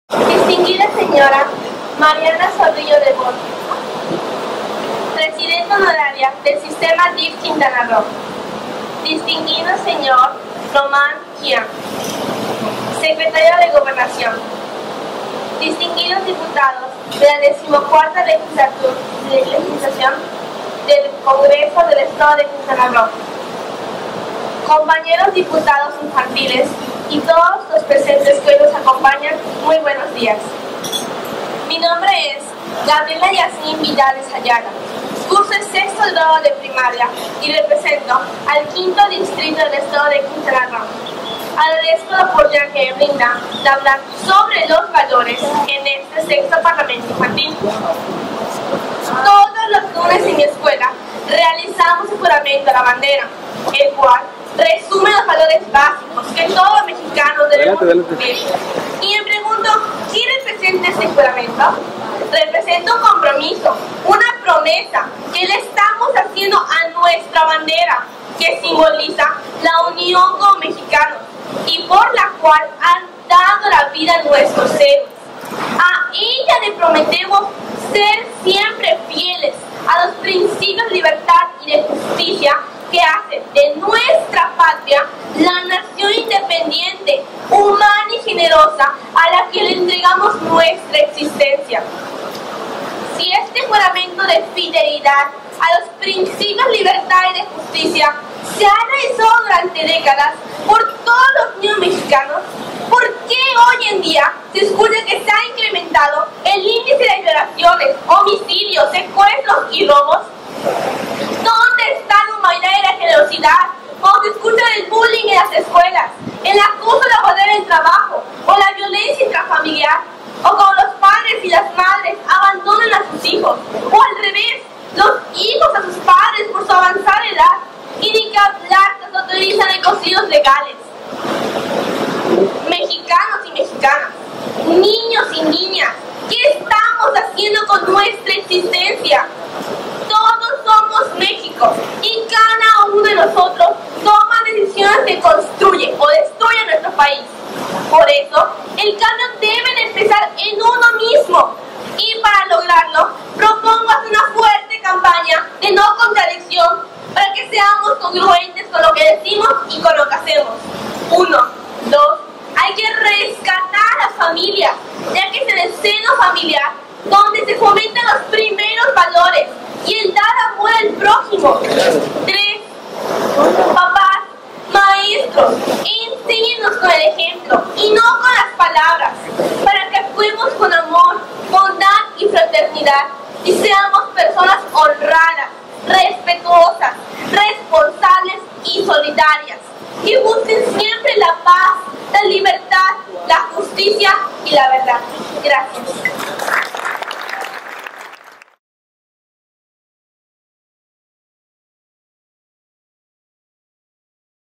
Diputada Infantil Gabriela Jasmin Vidales Ayala
Intervención de Gabriela Jasmin Vidales Ayala, diputada infantil por el distrito V. Tema: Recuperando los valores universales.